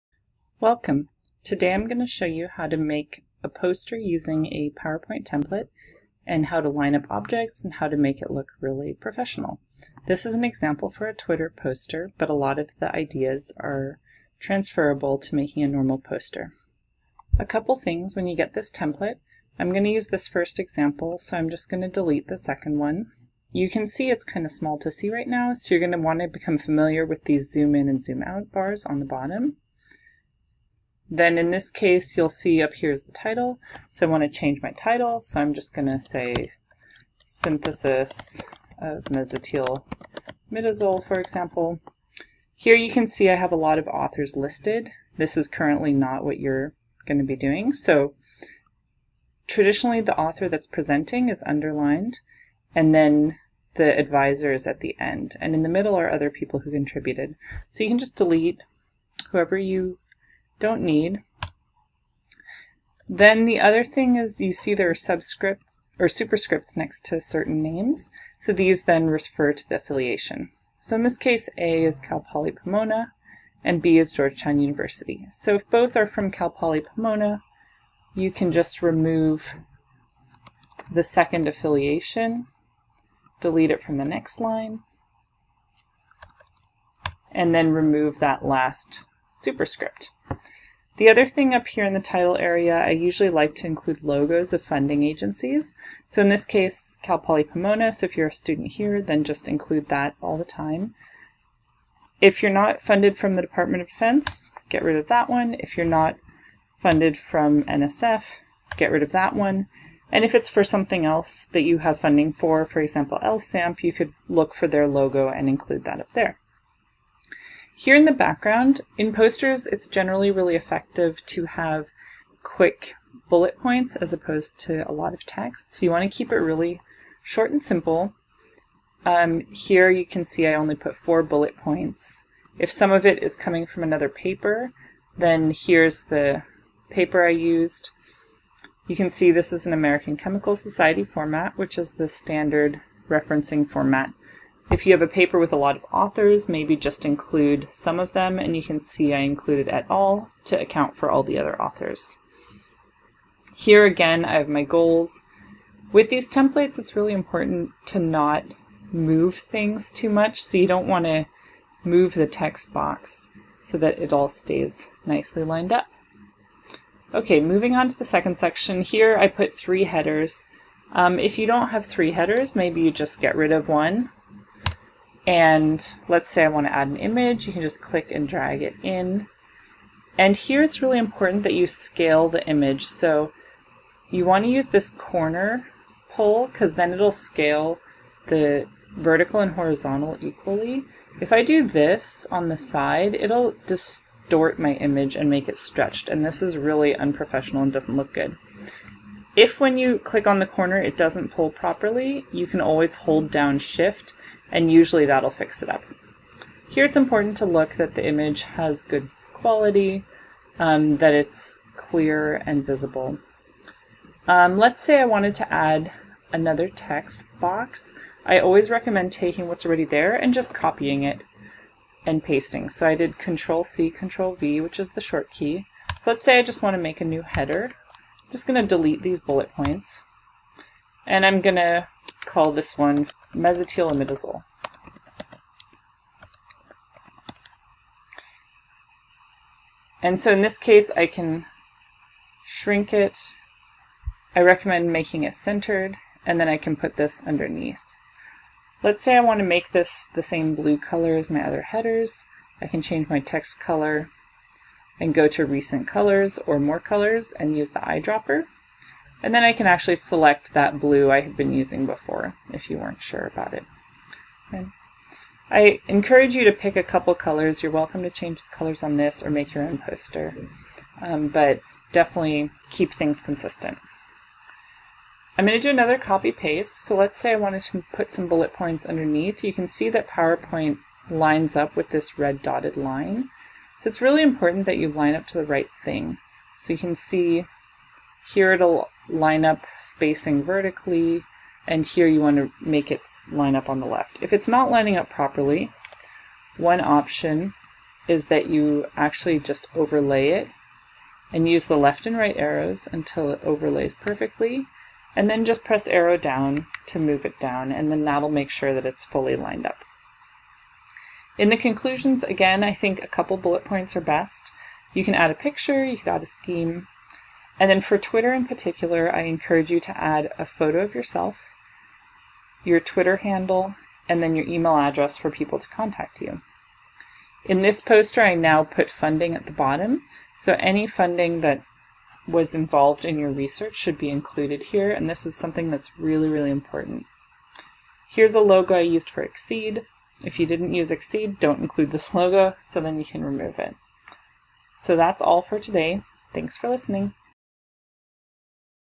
Using Powerpoint to Make a Poster from a Template
How to use a template in Powerpoint for making a scientific poster. This example is using a Twitter poster template.